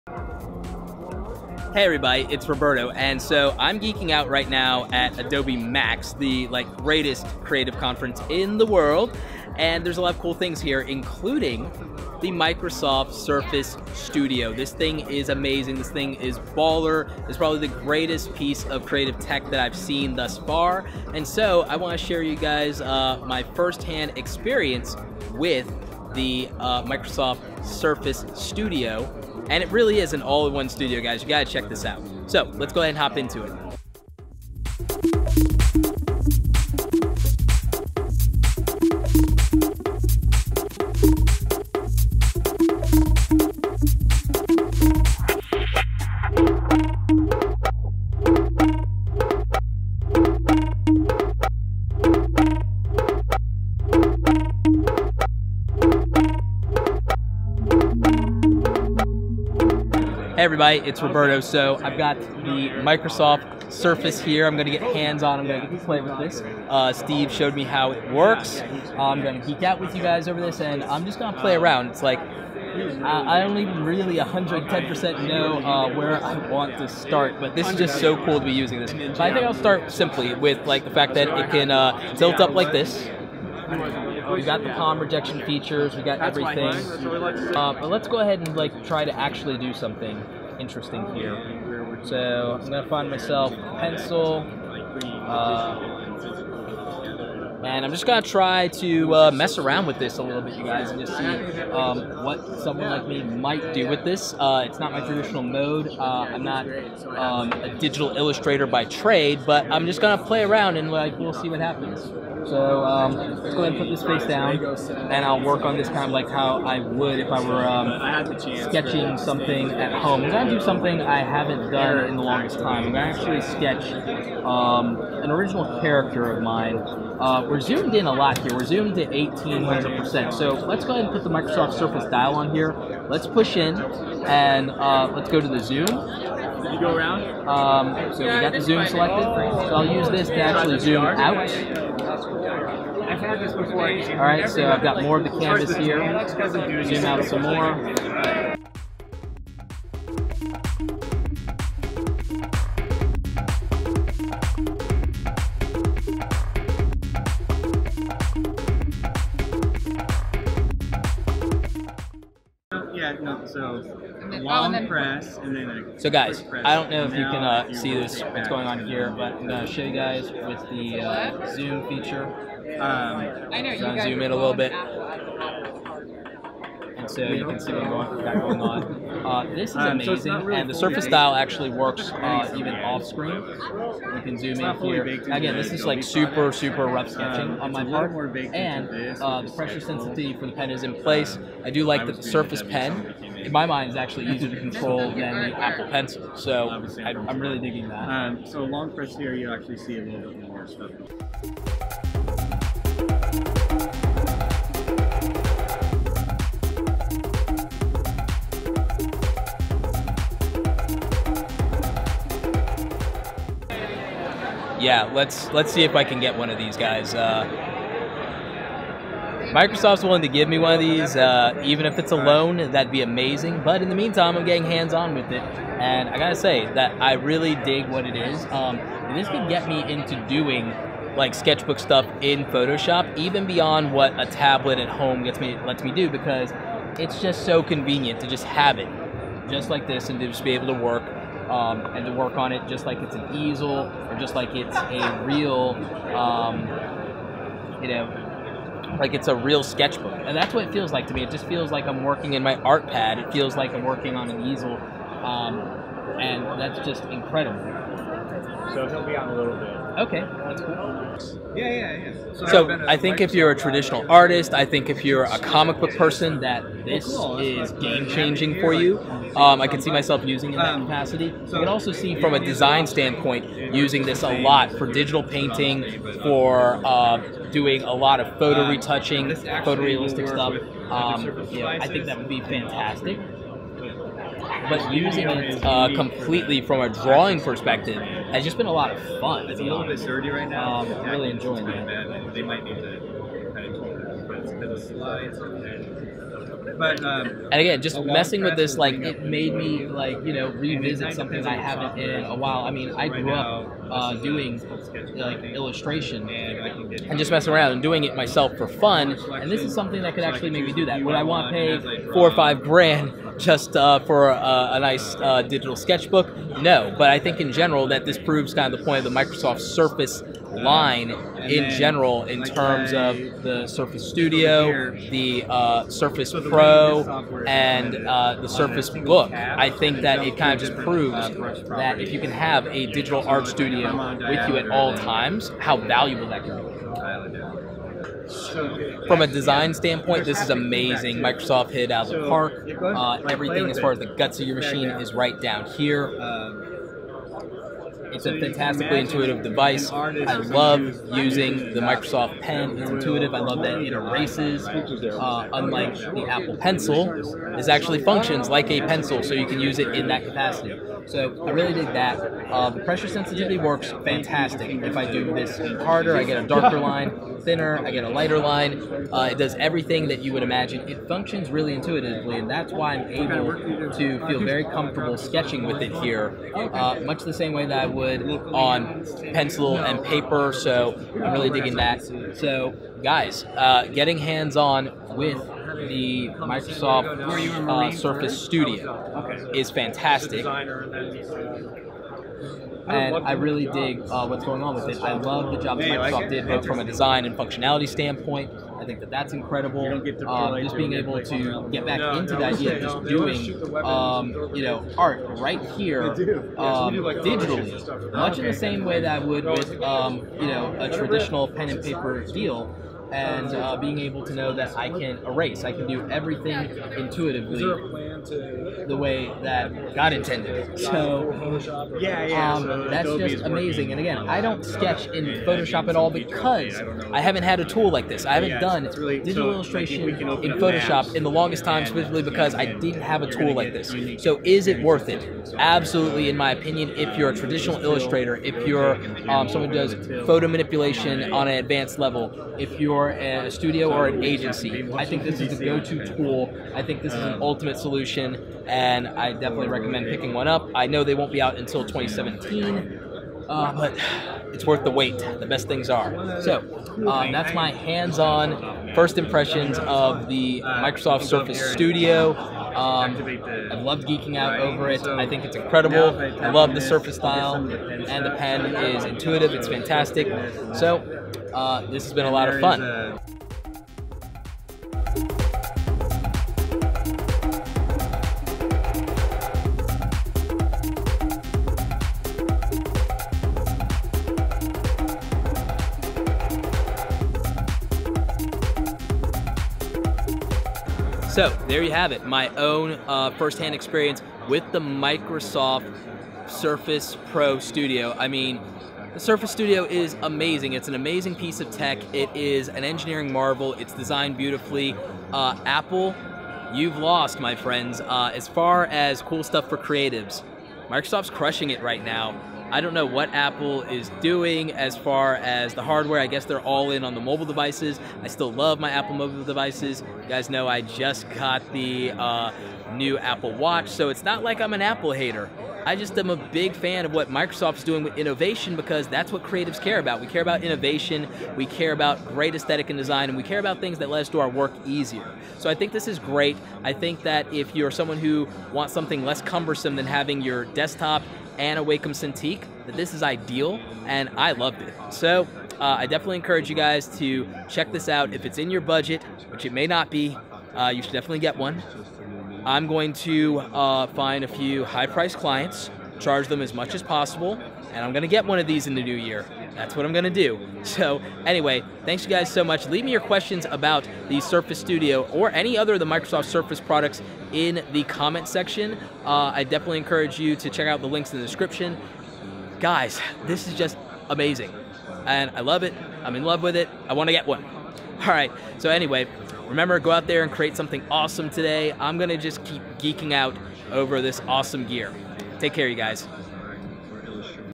Microsoft Surface Studio Hands On Review
Microsoft Surface Studio Hands On Review from Adobe MAX
The Microsoft Surface Studio and Surface Dial are a marvel of design that shows why the Microsoft Surface line is a real threat to Apple and their dominance among creatives. Here is my hands-on review of the Microsoft Surface Studio.

MICROSOFT SURFACE PRO 4

The Microsoft Surface Studio is great to use and the Surface Dial is one the most intuitive devices I've seen recently. Drawing on the Surface Studio felt really intuitive and if you are a digital artist or designer it will feel like using a Wacom Tablet but you get the benefit of an eraser built in.

For me not being a digital illustrator specifically it felt great in the hand, the palm rejection worked flawlessly and it didn't feel like there was a delay at all.

Overall the Microsoft Surface Studio is in my opinion the ideal Desktop for a creative professional.

MY YOUTUBE EQUIPMENT AND GEAR